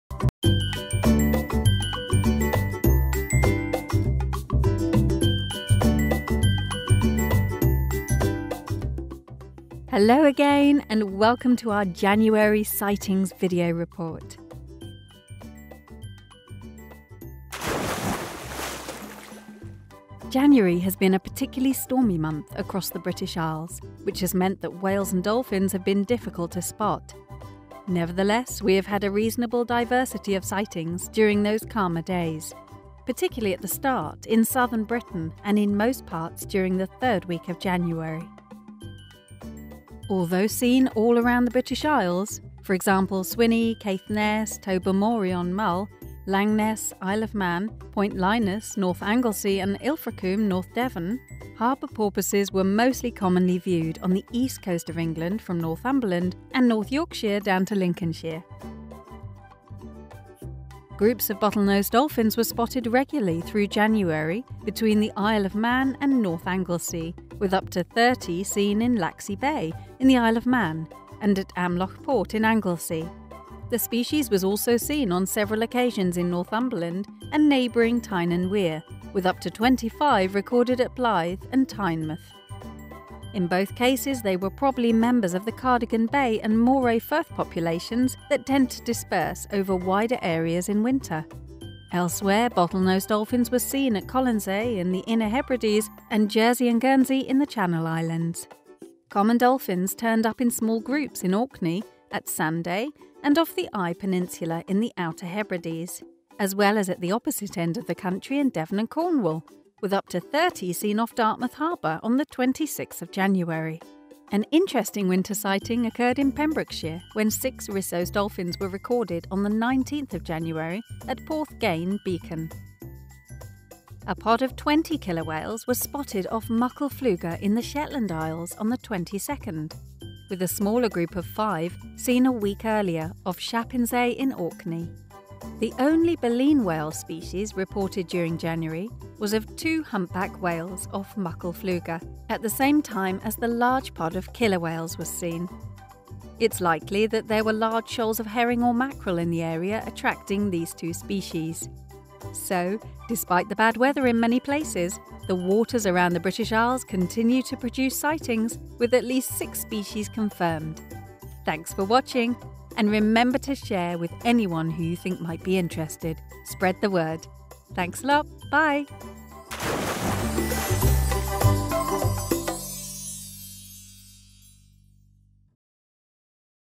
UK Whale & Dolphin Sightings January 2020
We're happy to present our sightings of whales, dolphins and porpoise from around the UK during January. Many thanks to our supporters and spotters for their continued incoming data.